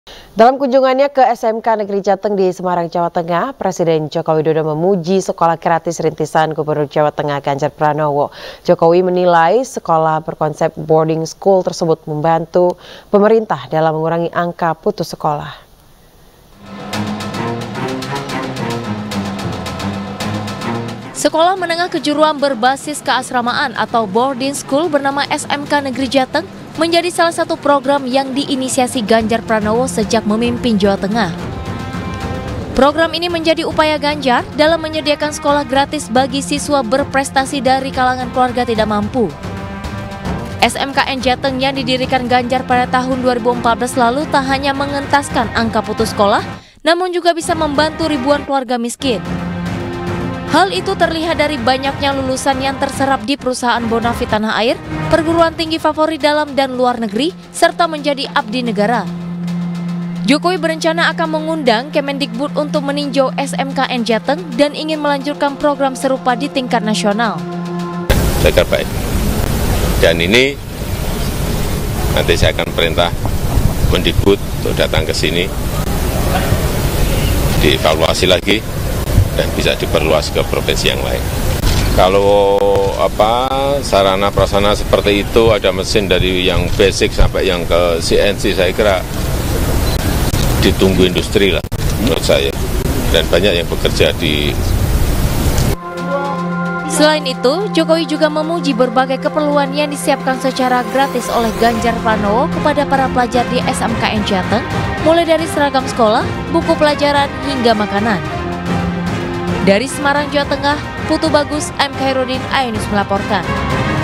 Jokowi Puji Inisiatif Bacapres Perindo Ganjar Pranowo Bangun SMKN Jateng Gratis
Presiden Joko Widodo (Jokowi) memberikan pujian terhadap program sekolah gratis SMKN Jawa Tengah (Jateng) yang digagas Gubernur Jateng, Ganjar Pranowo.

Program SMKN tersebut ditujukan untuk siswa-siswa dari keluarga tidak mampu. Siswa-siswa pun merasa bangga bisa belajar di institusi tersebut.